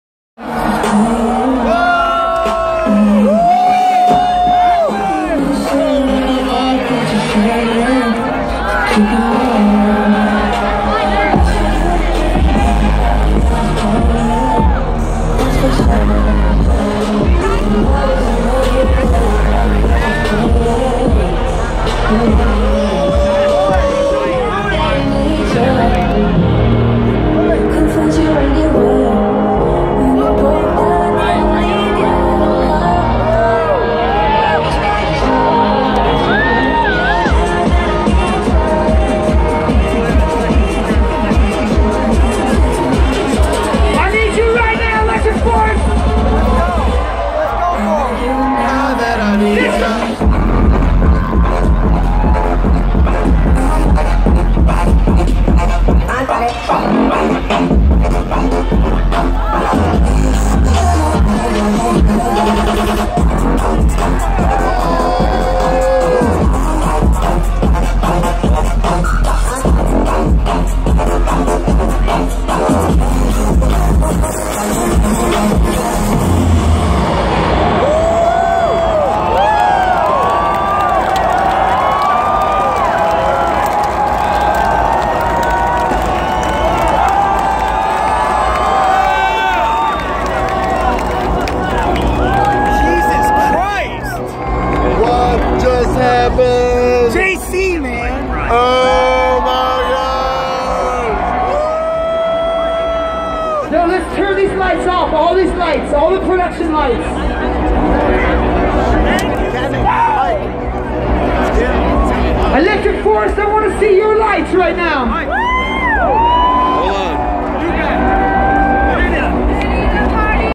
Skrillex - Electric Forest 2015 3/3
A short video of Skrillex closing out the Ranch Arena for the night at Electric Forest Festival in Rothbury, Michigan. Amazing.